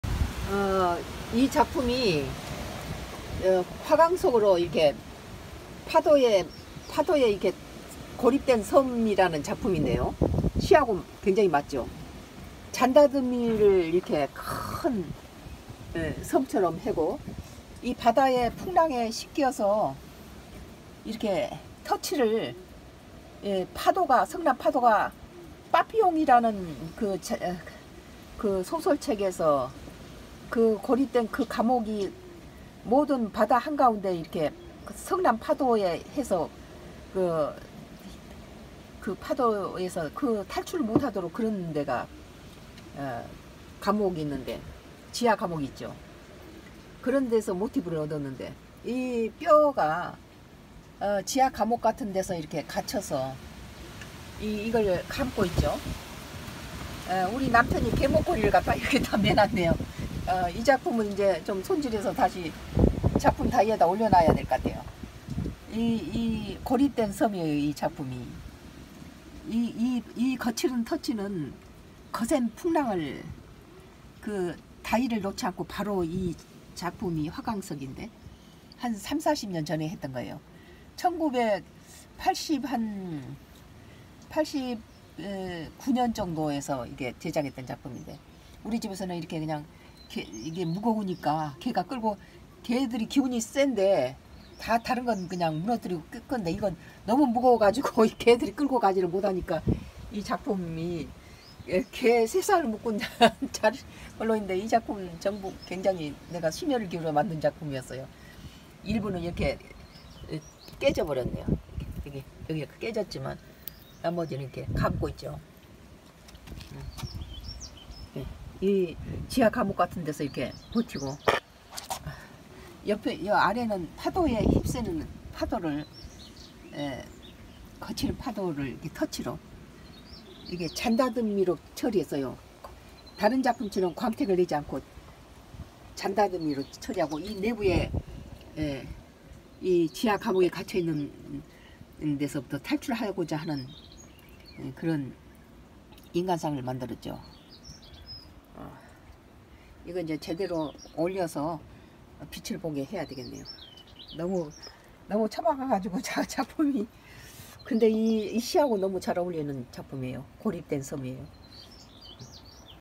( 시 ) 고립된 한개의 섬~~~~!@sungodcross
( 시 ) 고립된 한개의 섬 !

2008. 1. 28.

by sungodcross

바윗돌에 싸여진 고립된 섬 하나~

파도는 성난 거치른 숨소리로~

자갈과 모래로 섬을 때렸다~

하늘에 태양이 떠오르고

파도가 출렁거리며 달려와도~

바윗돌에 쌓인 섬은~

전복따는 아낙보며 미소짓네~

석양에 넘어가는 태양이

이별을 고하며

끼억 끼억 목메어

울고갈지라도~

고립된 섬하나~

그는 다시 떠오를 것을

믿는다네~

거대한 신의 손바닥 위에~

풍랑이 일고 ~

신의 입김을 불어서

커다란 배가 낙엽처럼 부서지네~

그러나~

신의 입김은 ~

고립된 섬을 넘어간다네~

이스라엘의  문설주에

어린양의 피처럼~@sungodcross